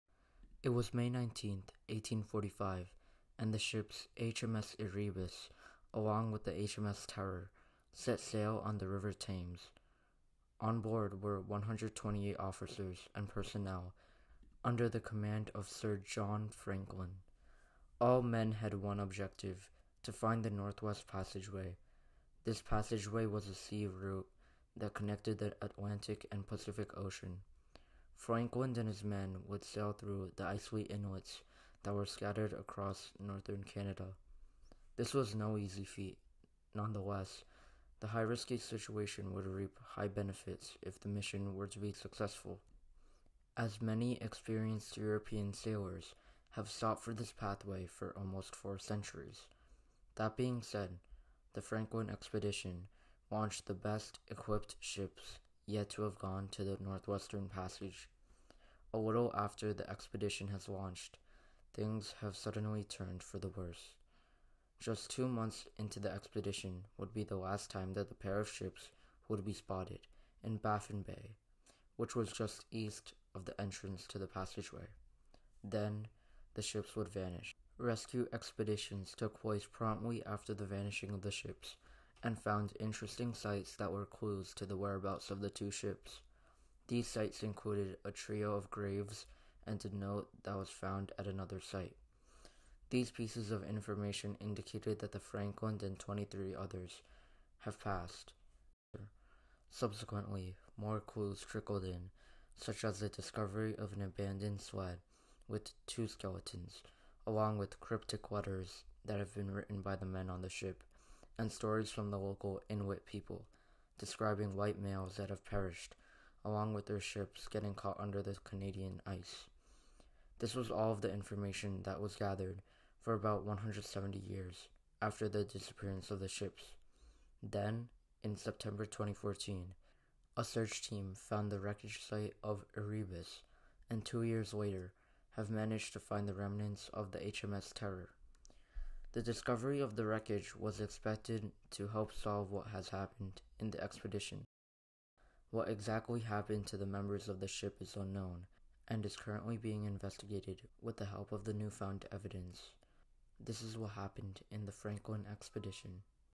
What Happened to the Franklin Expedition?
Hello friends! Today we will be looking at what happened in the Franklin Expedition! If you enjoyed watching this video, please consider liking and sharing the video.